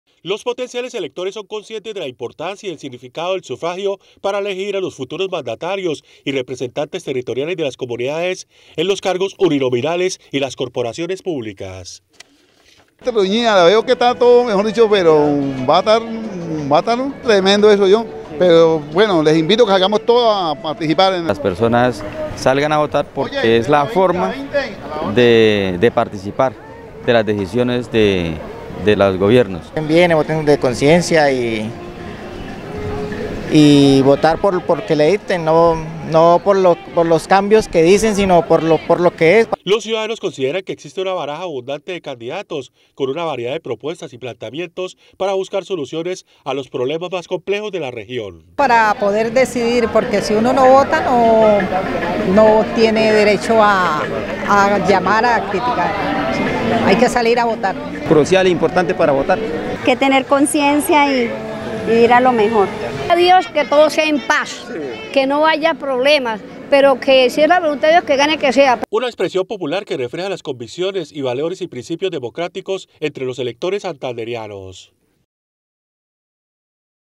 Mayoría de ciudadanos en Bucaramanga votará | Oro Noticias
Mayoría de ciudadanos consultados por 𝗢𝗿𝗼 𝗡𝗼𝘁𝗶𝗰𝗶𝗮𝘀 en Bucaramanga asegura que ejercerá su derecho al voto.